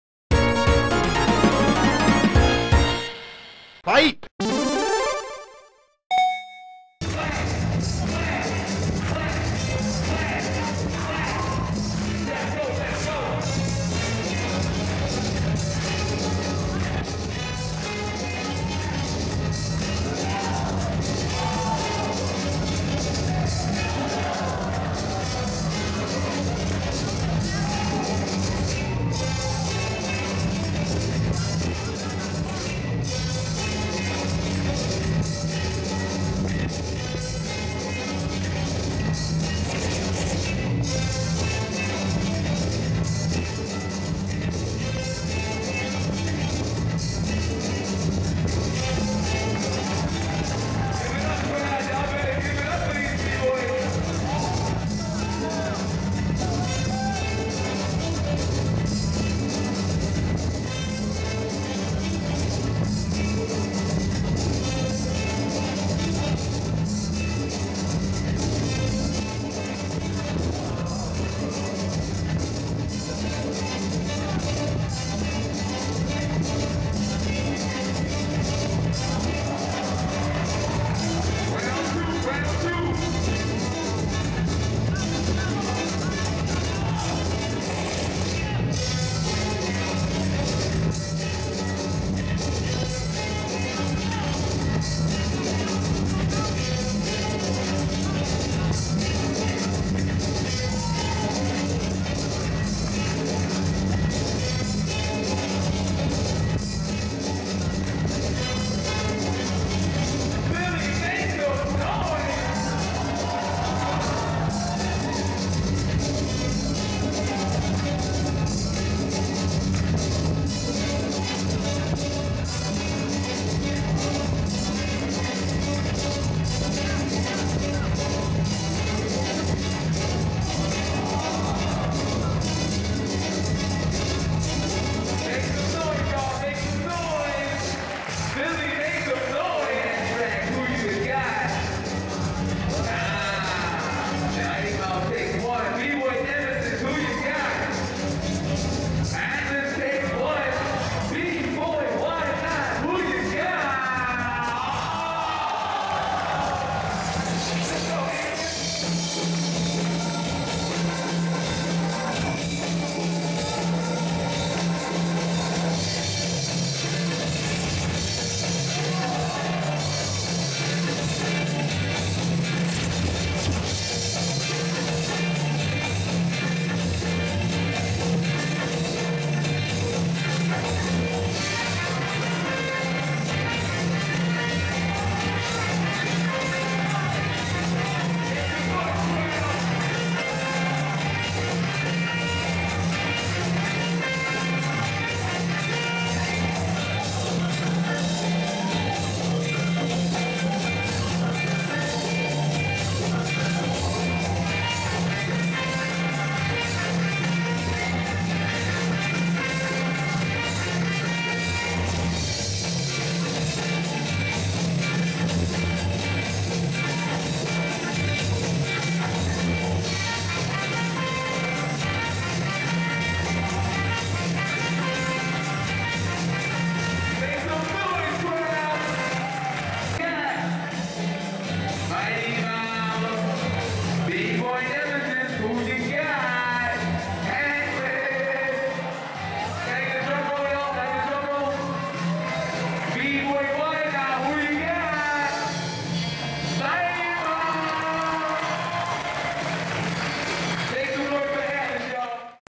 Atlas Vs Mighty Mouse | 1st Round | Red Bull BC One Philadelphia Cypher '14 | BNC X Strife
Red Bull BC One Philadelphia Cypher 2014

1st Round

Winner
Mighty Mouse (Repstyles)

Judges
Abstrak (Skill Methodz)
Y-Not (Rocksteady Crew)
Nemesis (Breaks Kru)

Emcee
Fame

DJ
Skeme Richards

Intro by
Jey Prime (Retro Flow)